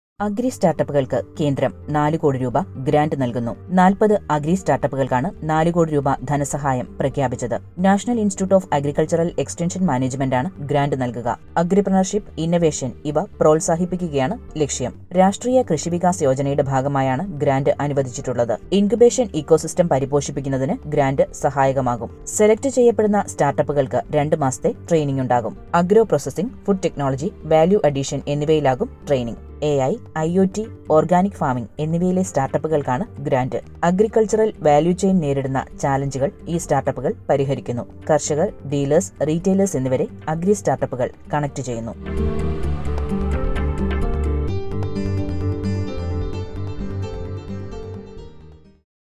Agri സ്റ്റാർട്ടപ്പുകൾക്ക് കേന്ദ്രം 4 കോടി രൂപ ഗ്രാന്റ് നൽകുന്നു | Government Providing Grant of 4 Cr
അഗ്രി സ്റ്റാർട്ടപ്പുകൾക്ക് കേന്ദ്രം 4 കോടി രൂപ ഗ്രാന്റ് നൽകുന്നു.
40 അഗ്രി സ്റ്റാർട്ടപ്പുകൾക്കാണ് 4 കോടി രൂപ ധനസഹായം പ്രഖ്യാപിച്ചത്.
National Institute of Agricultural Extension Management ആണ് ഗ്രാന്റ് നൽകുക.
Agripreneurship, ഇന്നവേഷൻ ഇവ പ്രോത്സാഹിപ്പിക്കുകയാണ് ലക്ഷ്യം.
Rashtriya Krishi Vikas Yojana യുടെ ഭാഗമായാണ് ഗ്രാന്റ് അനുവദിച്ചിട്ടുളളത്.
ഇൻകുബേഷൻ ഇക്കോസിസ്റ്റം പരിപോഷിപ്പിക്കുന്നതിന് ഗ്രാന്റ് സഹായകമാകും.
സെലക്ട് ചെയ്യപ്പെടുന്ന സ്റ്റാർട്ടപ്പുകൾക്ക് 2 മാസത്തെ ട്രെയിനിംഗ് ഉണ്ടാകും.
അഗ്രോ പ്രോസസിംഗ്, ഫു‍ഡ് ടെക്നോളജി, വാല്യു അഡീഷൻ എന്നിവയിലാകും ട്രെയിനിംഗ്.
AI, IoT, ഓർഗാനിക് ഫാമിംഗ് എന്നിവയിലെ സ്റ്റാർട്ടപ്പുകൾക്കാണ് ഗ്രാന്റ്.
അഗ്രിക്കൾച്ചർ വാല്യു ചെയിൻ നേരിടുന്ന ചാലഞ്ചുകൾ ഈ സ്റ്റാർട്ടപ്പുകൾ പരിഹരിക്കുന്നു.
കർഷകർ, ഡീലേഴ്സ്, റീട്ടെയ്ലേഴ്സ് എന്നിവരെ അഗ്രി സ്റ്റാർട്ടപ്പുകൾ കണക്റ്റ് ചെയ്യുന്നു.